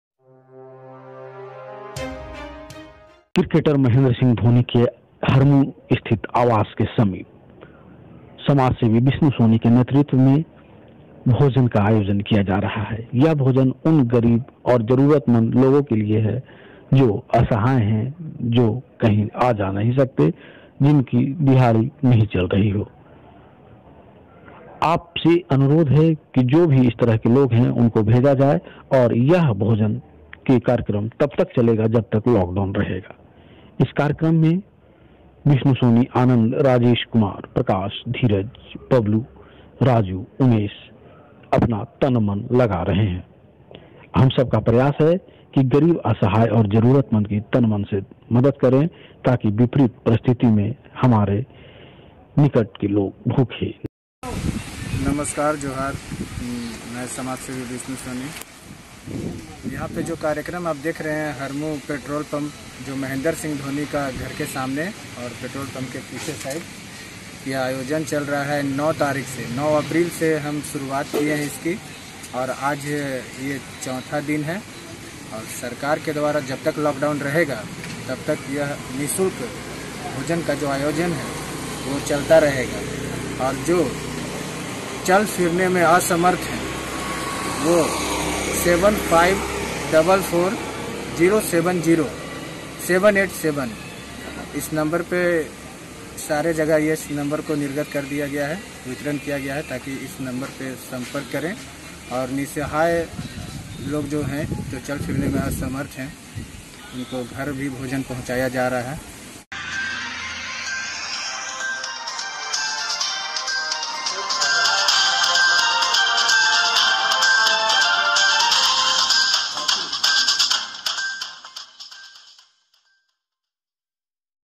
हरमू पेट्रोल पंप के पीछे भूखे और जरूरतमंद के लिए भोजन की व्यवस्था-समाजसेवी विष्णु सोनी
राँची।। समाजसेवी विष्णु सोनी के नेतृत्व में हरमू महेंद्र सिंह धोनी घर के सामने पेट्रोल पंप के पीछे मैं भोजन का आयोजन किया जा रहा है। जब तक लॉक डाउन रहेगा तब तक यह आयोजन निरंतर चलता रहेगा। आप सभी से नम्र निवेदन है कि हरमू क्षेत्र के जो भूखे और जरूरतमंद हैं जिन्हें भोजन की अति आवश्यकता है उन्हें आप यहां भेजने की कष्ट करें। इस कार्यक्रम में कर्मठ सहयोगी विष्णु सोनी ,आनंद ,राजेश कुमार सिंह,  प्रकाश दा, धीरज कुमार सिंह ,बबलू साहू ,राजू कुमार झा ,उमेश साहू। तन मन धन से सारे लोग सेवा दे रहे हैं।
हम सब का प्रयास है कि गरीब असहाय और जरूरतमंद की तन मन धन से मदद करना ताकि इस विपरीत परिस्थिति में हमारे भाई बहन भूखे ना रहे और इस विपरीत परिस्थिति में डट कर मुकाबला करें और एक स्वस्थ भारत का निर्माण करें।